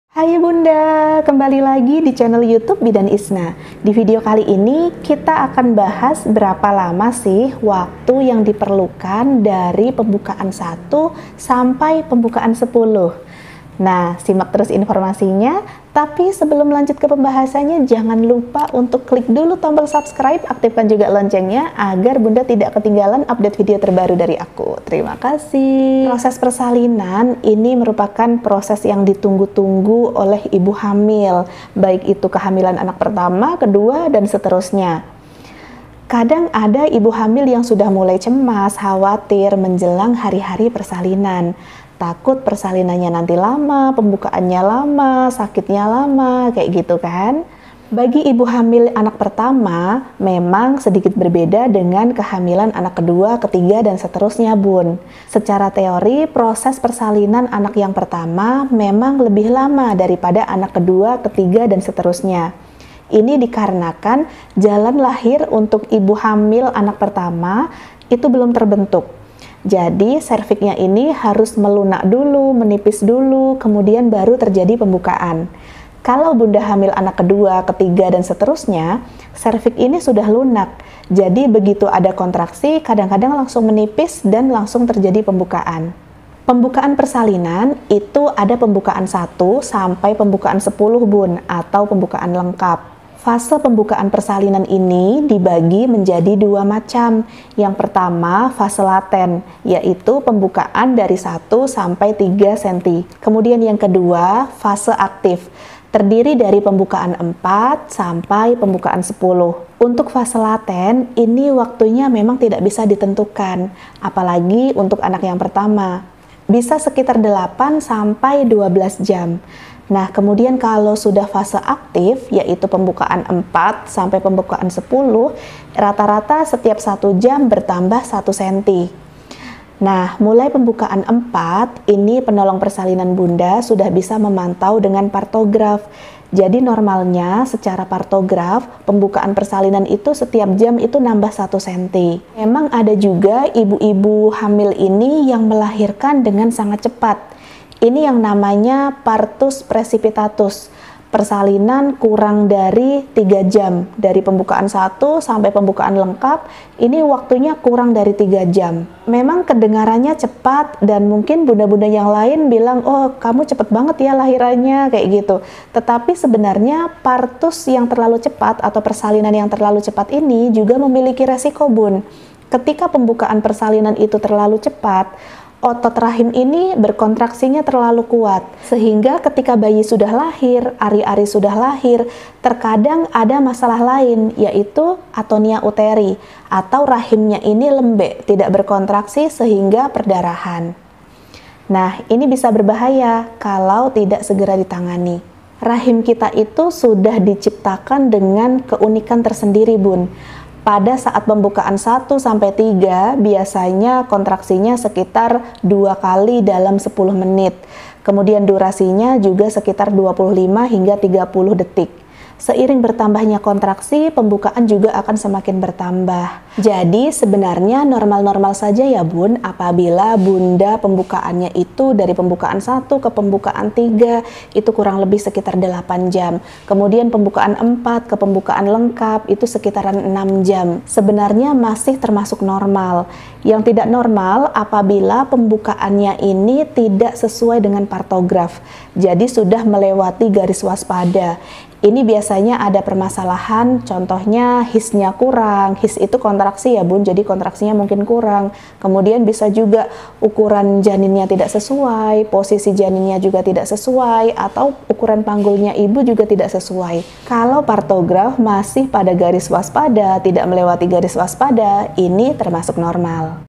BERAPA LAMA PEMBUKAAN 1 SAMAPAI 10 PERSALINAN SAMPAI BAYI LAHIR, BIDAN ISNA
hai bunda selamat datang di channel YouTube @bidanisna di video ini kita bahas berapa lama pembukaan persalinan dari 1 sampai 10 dan kapan bayi akan lahir,

bagi bunda ibu hamil yang sudah mendekati hpl sering sekali merasakan khawatir kapan bayi akan lahir, berapa lama proses persalinan, bunda bisa simak informasi berikut ini jangan lupa untuk like komen dan subscribe terimakasih